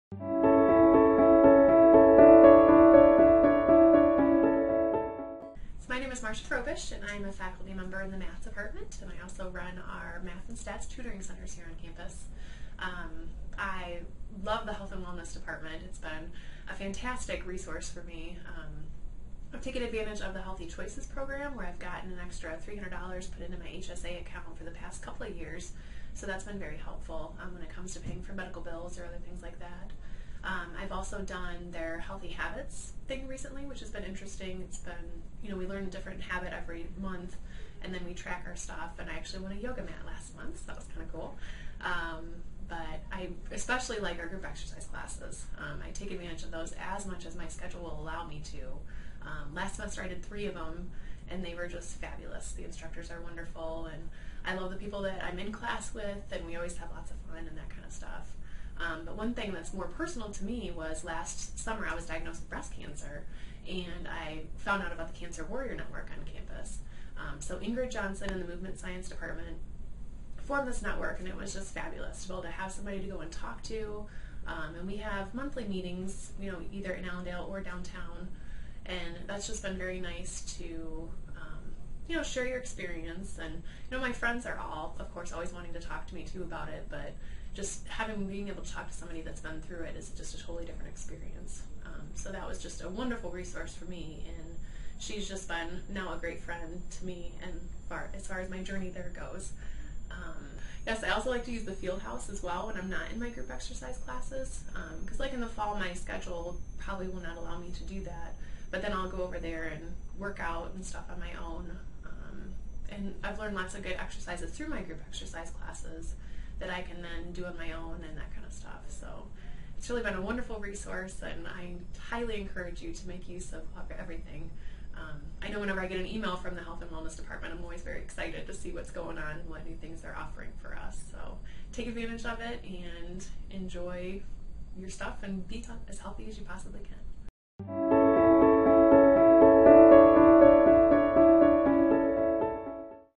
It's What We Do: Marcia Frobish
GVSU's Health and Wellness program offers health resources and programming for faculty and staff. This testimony from Marcia Frobish is one of many success stories of people who have been through the program.